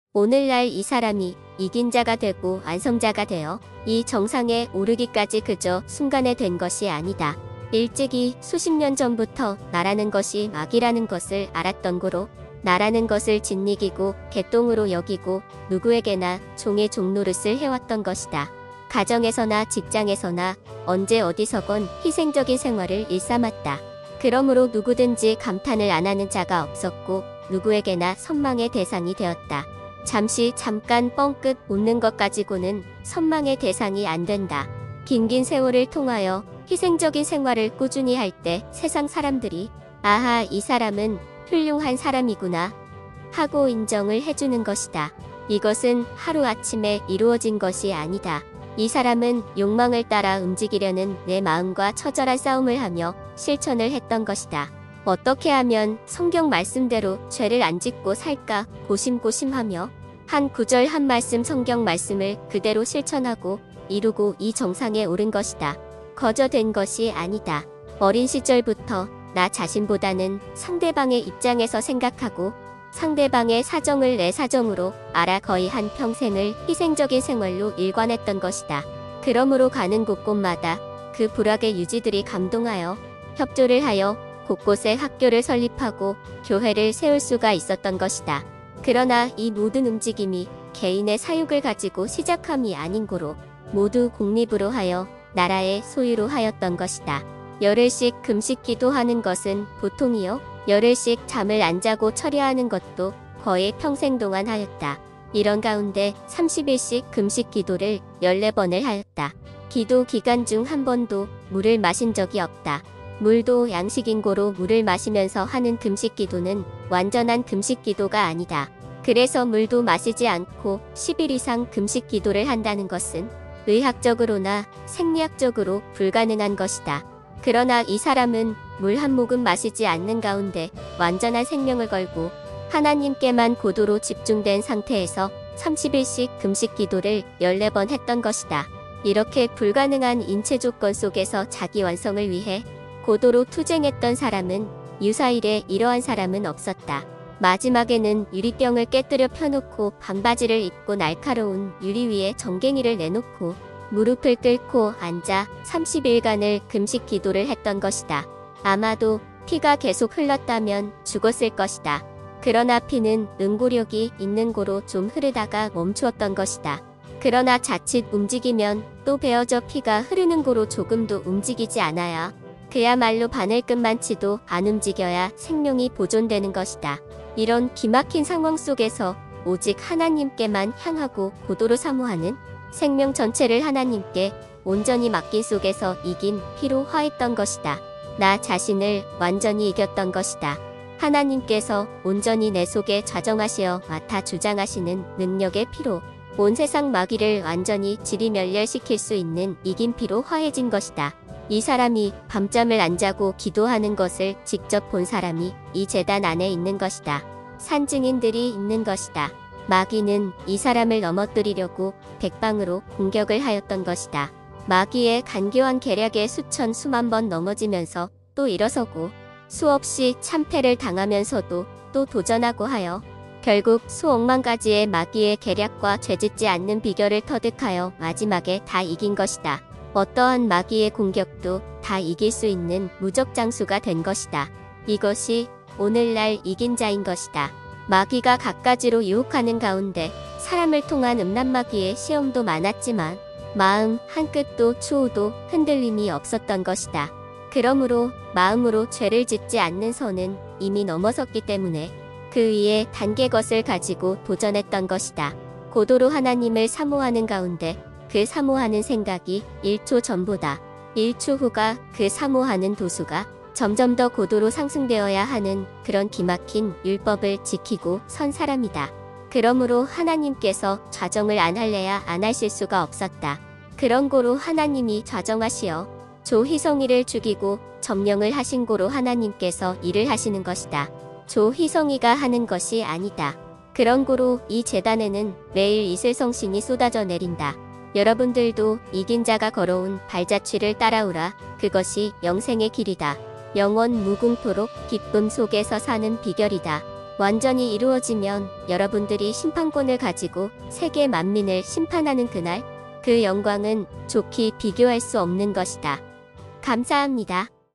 열흘씩 금식기도하는 것은 보통이요, 열흘씩 잠을 안자고 철야하는 것도 거의 평생동안 하였다 이런 가운데 30일씩 금식기도를 14번을 하였다. 기도기간중 한번도 물을 마신 적이 없다
오늘날 이 사람이 이긴자가 되고 완성자가 되어 이 정상에 오르기까지 그저 순간에 된 것이 아니다. 일찍이 수십년 전부터 ‘나’라는 것이 마귀라는 것을 알았던고로 ‘나’라는 것을 짓이기고 개똥으로 여기고 누구에게나 종의 종노릇을 해왔던 것이다. 가정에서나 직장에서나 언제 어디서건 희생적인 생활을 일삼았다. 그러므로 누구든지 감탄을 안하는 자가 없었고 누구에게나 선망의 대상이 되었다. 잠시 잠깐 뻥끗 웃는 것 가지고는 선망의 대상이 안된다. 긴긴 세월을 통하여 희생적인 생활을 꾸준히 할 때 세상 사람들이 ‘아하’ 이 사람은 휼륭한 사람이구나! 하고 인정을 해주는 것이다. 이것은 하루 아침에 이루어진 것이 아니다.

이 사람은 욕망을 따라 움직이려는 내 마음과 처절한 싸움을 하며 실천을 했던 것이다. 어떻게 하면 성경 말씀대로 죄를 안짓고 살까 고심고심하며 한 구절 한 말씀 성경 말씀을 그대로 실천하고, 이루고 이 정상에 오른 것이다. 거저 된 것이 아니다.
어린 시절부터 나 자신보다는 상대방의 입장에서 생각하고 상대방의 사정을 내 사정으로 알아 거의 한 평생을 희생적인 생활로 일관했던 것이다. 그러므로 가는 곳곳마다 그 부락의 유지들이 감동하여
협조를 하여 곳곳에 학교를 설립하고 교회를 세울 수가 있었던 것이다.
그러나 이 모든 움직임이 개인의 사욕을 가지고 시작함이 아닌고로
모두 공립으로 하여 나라의 소유로 하였던 것이다.
열흘씩 금식기도하는 것은 보통이요, 열흘씩 잠을 안자고 철야하는 것도 거의 평생동안 하였다.
이런 가운데 30일씩 금식기도를 14번을 하였다. 기도 기간중 한 번도 물을 마신 적이 없다.
물도 양식인고로 물을 마시면서 하는 금식기도는 완전한 금식기도가 아니다. 그래서 물도 아시지 않고 10일 이상 금식기도를 한다는 것은
의학적으로나 생리학적으로 불가능한 것이다.

그러나 이 사람은 물 한 모금 마시지 않는 가운데 완전한 생명을 걸고
하나님께만 고도로 집중된 상태에서 30일씩 금식기도를 14번 했던 것이다.
이렇게 불가능한 인체조건 속에서 자기완성을 위해
고도로 투쟁했던 사람은 유사이래 이러한 사람은 없었다.
마지막에는 유리병을 깨뜨려 펴놓고 반바지를 입고 날카로운 유리위에 정갱이를 내놓고 무릎을 끓고 앉아 30일간을 금식기도를 했던 것이다. 아마도 피가 계속 흘렀다면 죽었을 것이다.
그러나 피는 응고력이 있는고로 좀 흐르다가 멈추었던 것이다.
그러나 자칫 움직이면 또 베어져 피가 흐르는고로 조금도 움직이지 않아야,
그야말로 바늘끝만치도 안움직여야 생명이 보존되는 것이다.
이런 기막힌 상황 속에서 오직 하나님께만 향하고 고도로 사모하는,
생명 전체를 하나님께 온전히 맡긴 속에서 이긴 피로 화했던 것이다.
나 자신을 완전히 이겼던 것이다.

하나님께서 온전히 내속에 좌정하시어 맡아 주장하시는 능력의 피로 온 세상 마귀를 완전히 지리멸렬 시킬 수 있는 이긴피로 화해진 것이다.
이 사람이 밤잠을 안자고 기도하는 것을 직접 본 사람이 이 제단 안에 있는 것이다.
산 증인들이 있는 것이다. 마귀는 이 사람을 넘어뜨리려고 백방으로 공격을 하였던 것이다.
마귀의 간교한 계략에 수천 수 만번 넘어지면서 또 일어서고 수 없이 참패를 당하면서도 또 도전하고 하여 결국 수 억만가지의 마귀의 계략과 죄짓지 않는 비결을 터득하여 마지막에 다 이긴 것이다.
어떠한 마귀의 공격도 다 이길 수 있는 무적장수가 된 것이다. 이것이 오늘날 이긴자인 것이다. 마귀가 갖가지로 유혹하는 가운데 사람을 통한 음란마귀의 시험도 많았지만 마음 한끝도 추호도 흔들림이 없었던 것이다.
그러므로 마음으로 죄를 짓지 않는 선은 이미 넘어섰기 때문에
그 위의 단계 것을 가지고 도전했던 것이다.
고도로 하나님을 사모하는 가운데 그 사모하는 생각이 일초 전보다
일초후가 그 사모하는 도수가 점점더 고도로 상승되어야 하는 그런 기막힌 율법을 지키고 선 사람이다.
그러므로 하나님게서 좌정을 안 할래야 안할실 수가 없었다. 그런고로 하나님이 좌정하시어 조 희성이를 죽이고 점령을 하신고로 하나님께서 일을 하시는 것이다. 조 희성이가 하는 것이 아니다. 그런고로 이 제단에는 매일 이슬성신이 쏟아져 내린다. 여러분들도 이긴자가 걸어온 발자취를 따라오라 그것이 영생의 길이다.
영원무궁토록 기쁨 속에서 사는 비결이다.
완전히 이루어지면 여러분들이 심판권을 가지고 세계 만민을 심판하는 그 날, 그 영광은 족히 비교할 수 없는 것이다.* 감사합니다